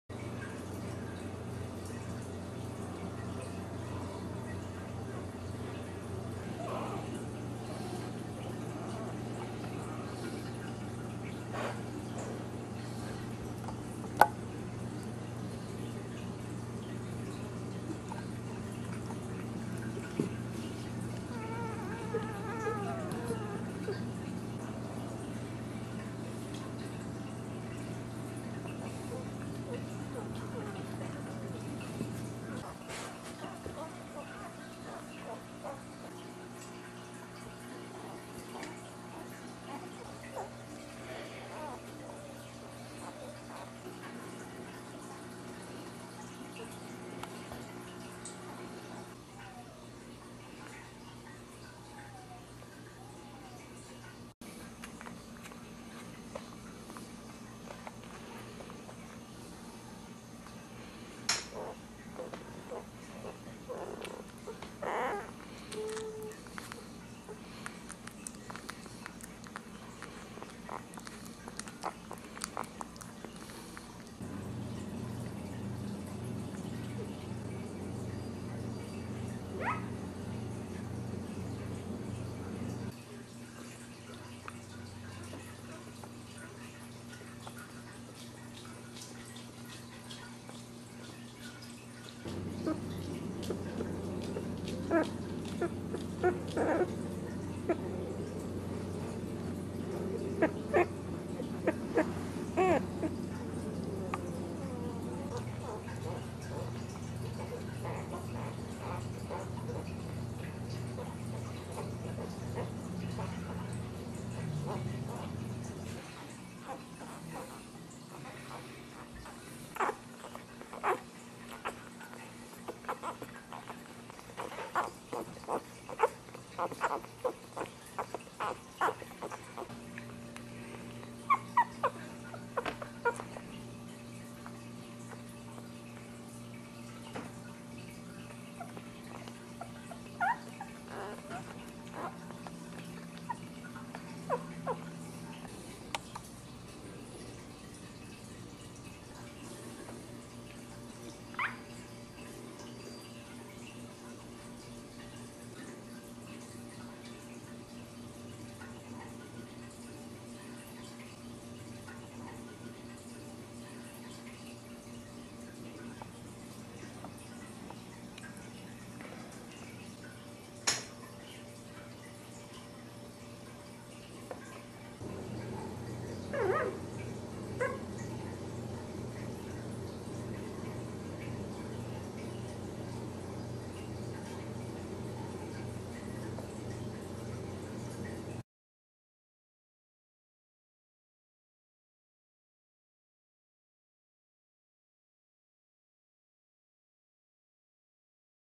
Cute 10 Day Old Puppies - Adorable Dachshund
Cutest Puppies who are just 10 days old... mom is still very protective and caring. She never lets them alone... it is so beautiful and cute to see!